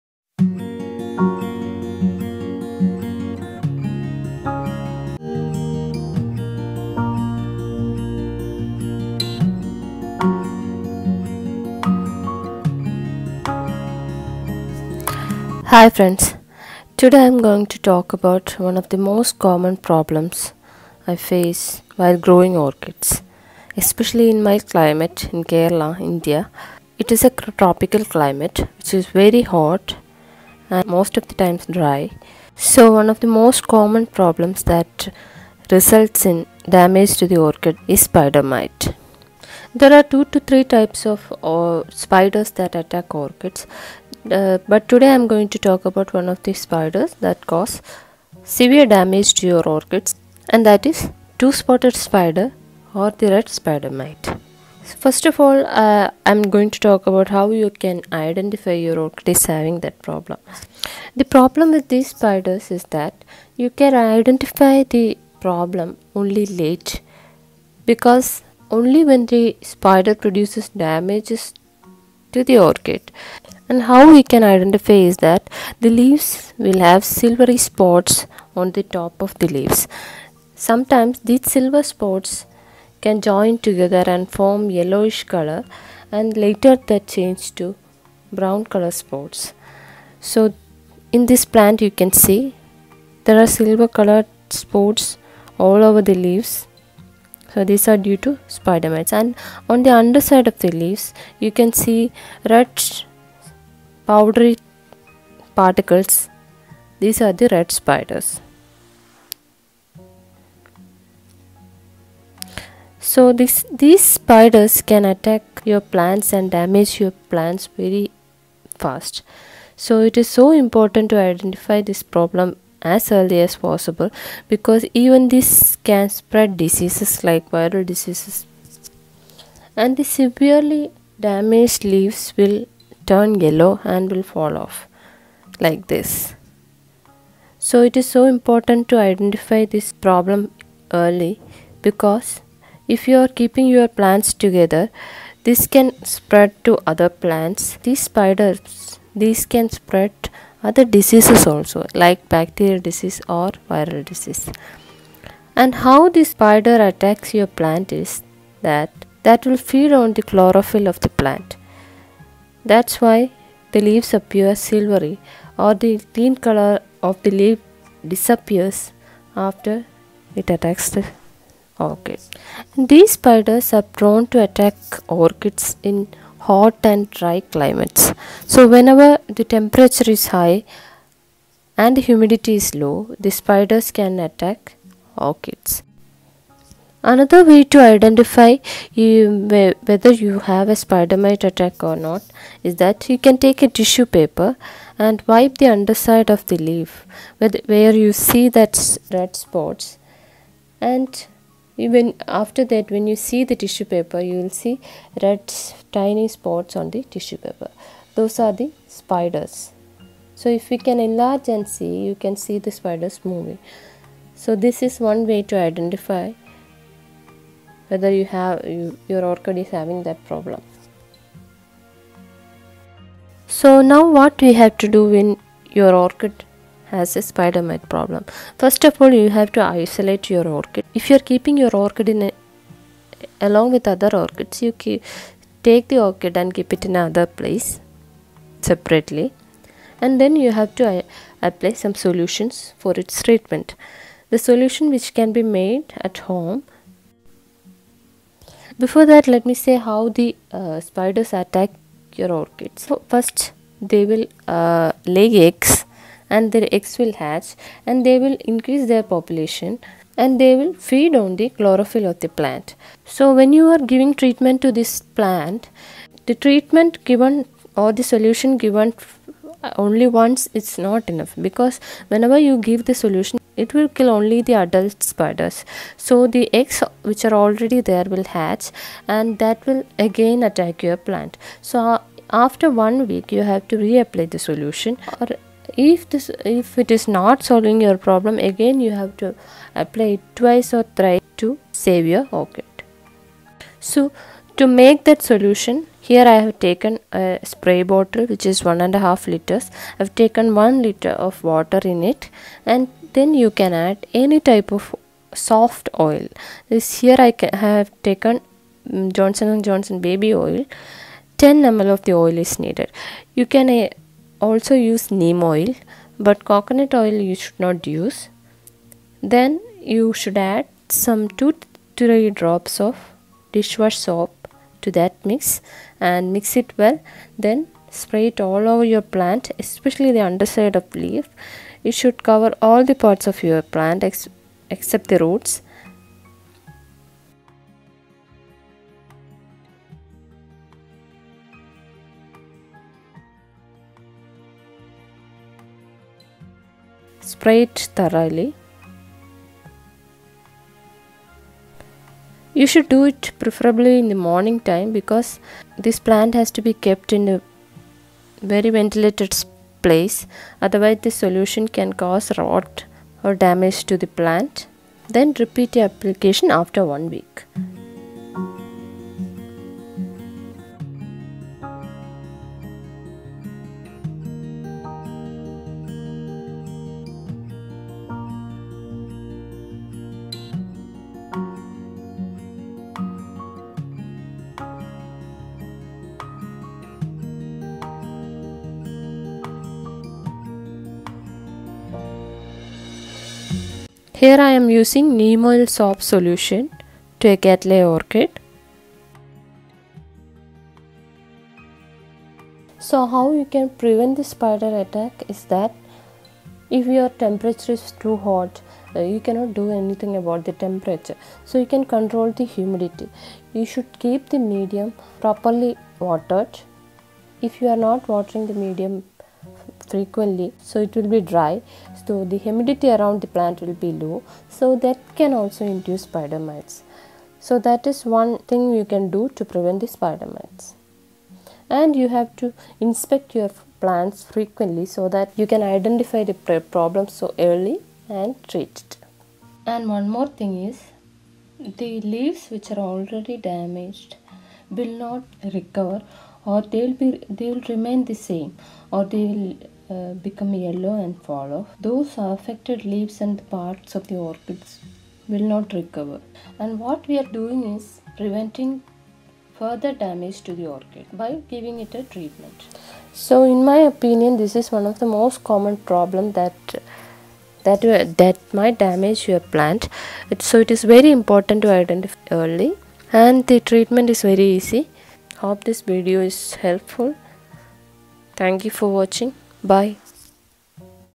SPIDERMITES in orchids-identify and rescue easily in English Ep:60
This video is about one of the most common problems I face while growing orchids and that is spidermites.The video shows how to identify spidermites in your orchids early and demonstrates an easy homemade solution for  it.

Other videos on Orchid disease and control

Snails in orchids

Sunburn in orchids

Using fungicide